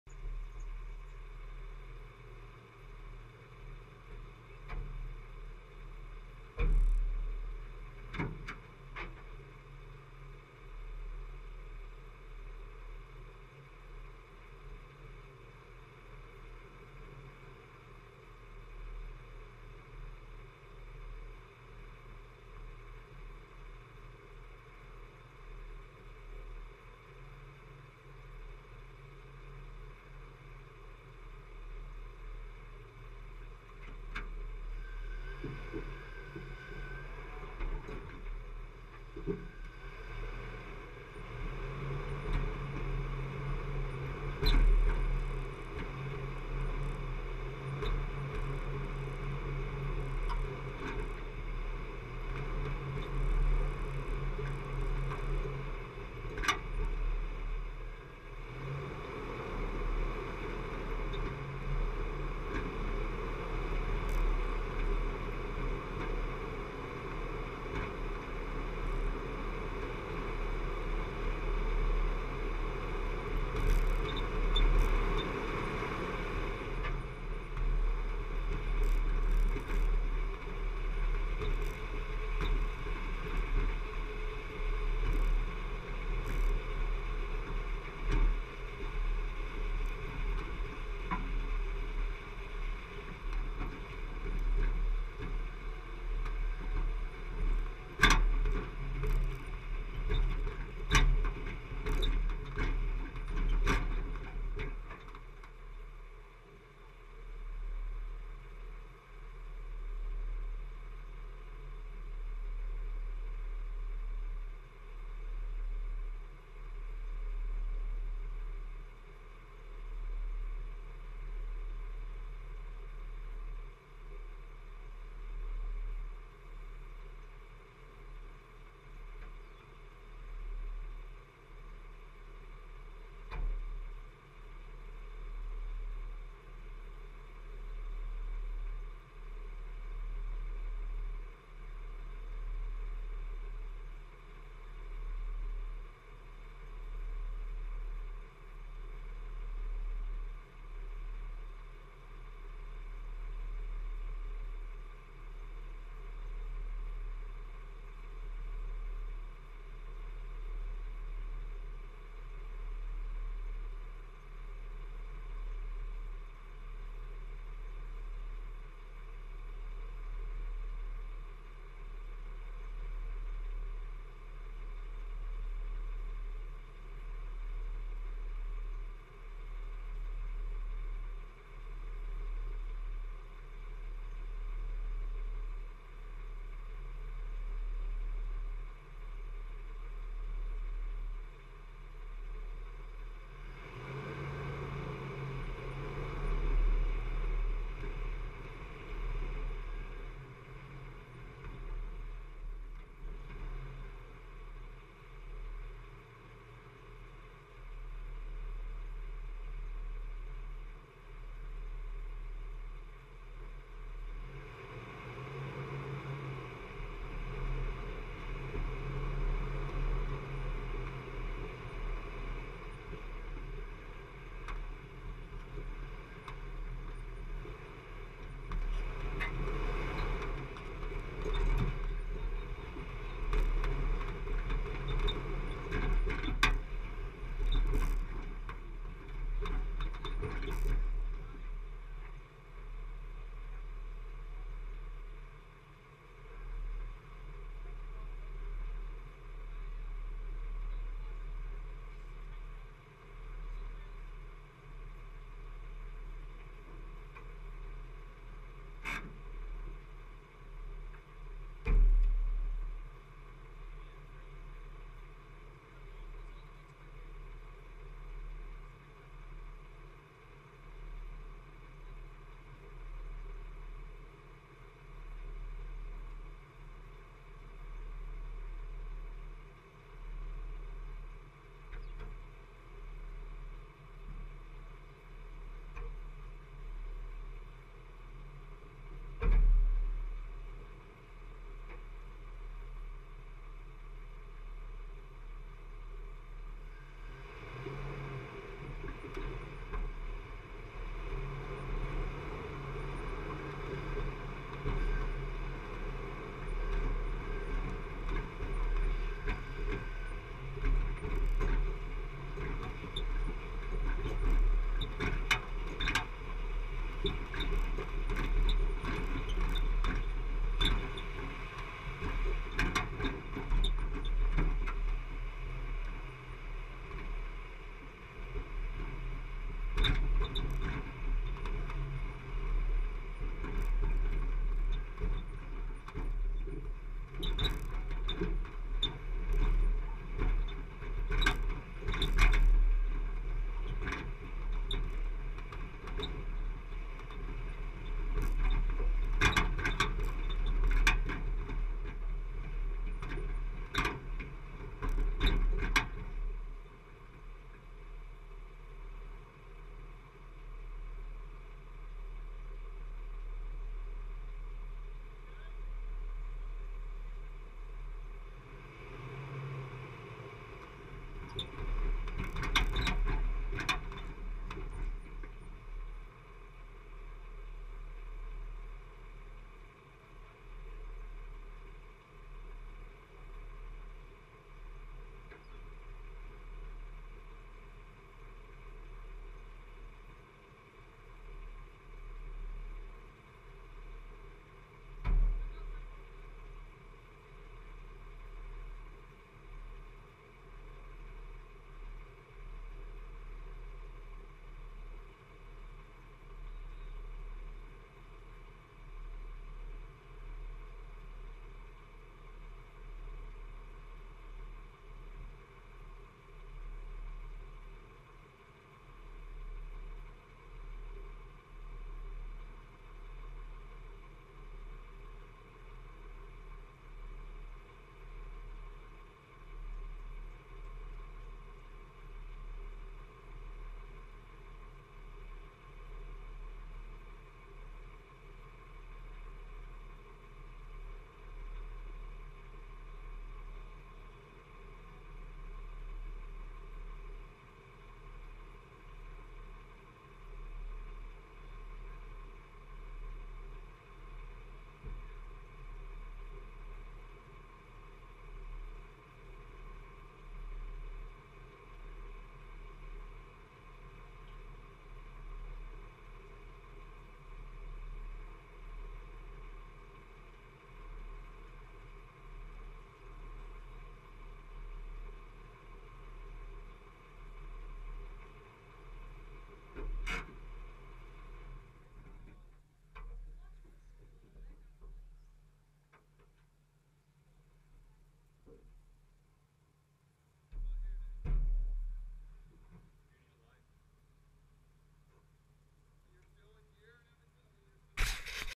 Hidden Falls GoPro Hero 3
This is a video of some of the guys from my group and I out in Marble Falls Tx.